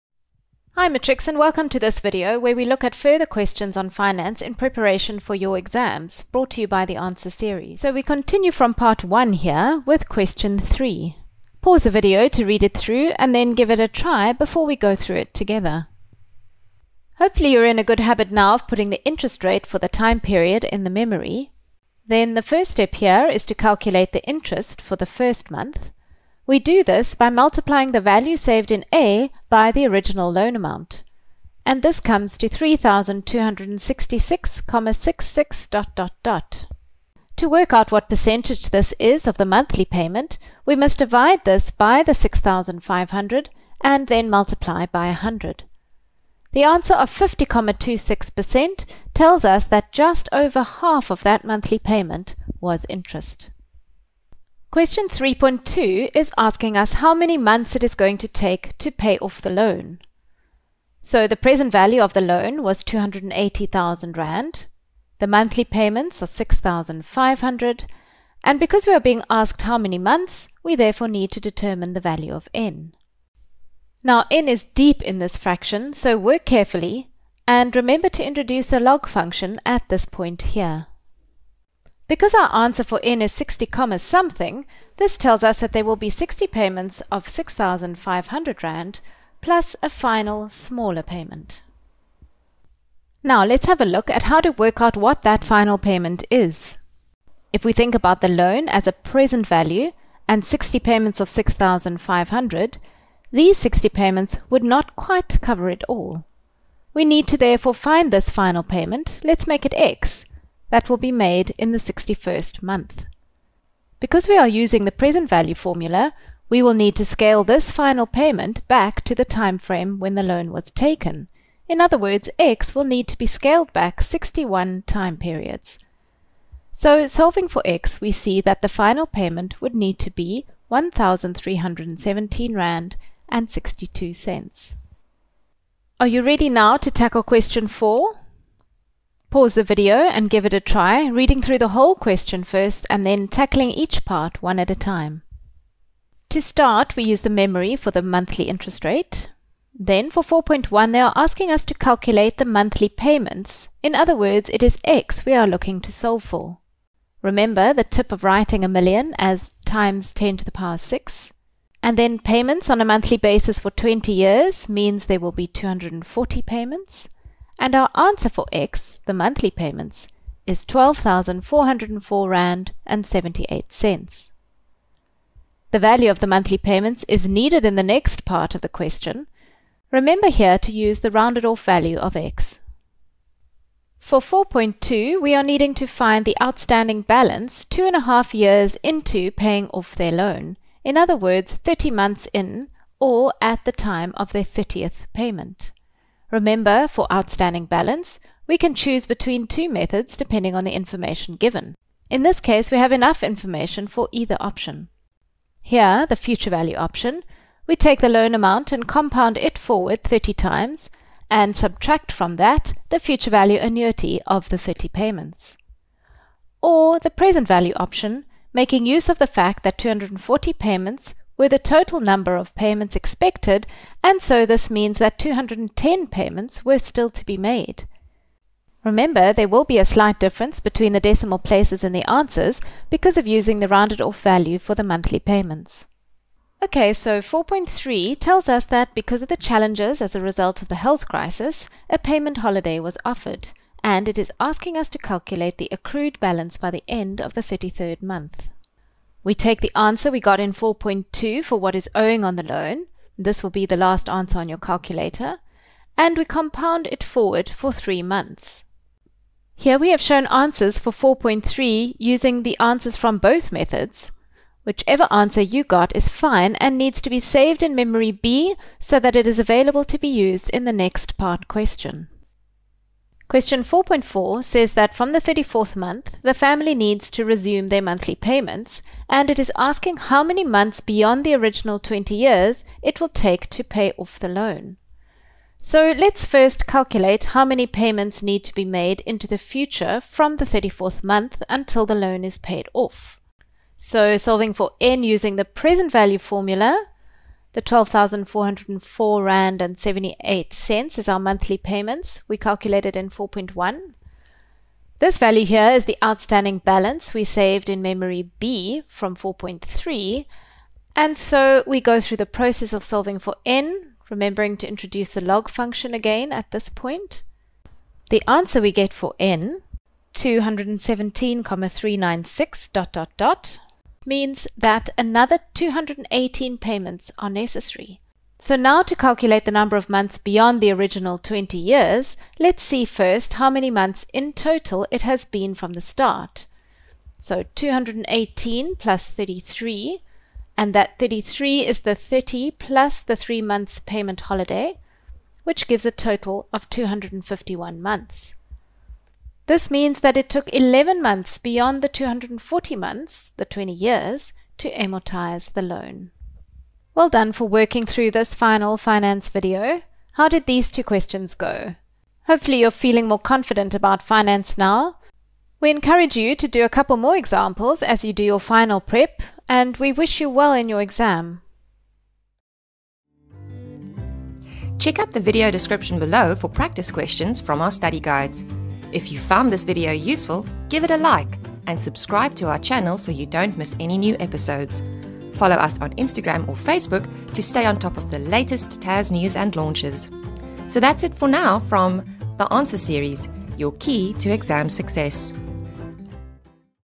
GRADE 12 MATHS FINANCE – Exam Prep (PART 2) #15
This video is a continuation of the important consolidation of skills by means of examination questions to improve on techniques.

Practice Questions for this video
Gr 12 Mathematics PAST PAPERS TOOLKIT
• Page 20: DBE 2017 P1 – Question 6
• Page 30: DBE 2019 P1 – Question 6
• Page 35: DBE 2020 P1 – Question 6
• Page 52: IEB 2019 P1 – Question 2
• Page 61: IEB 2020 P1 – Question 10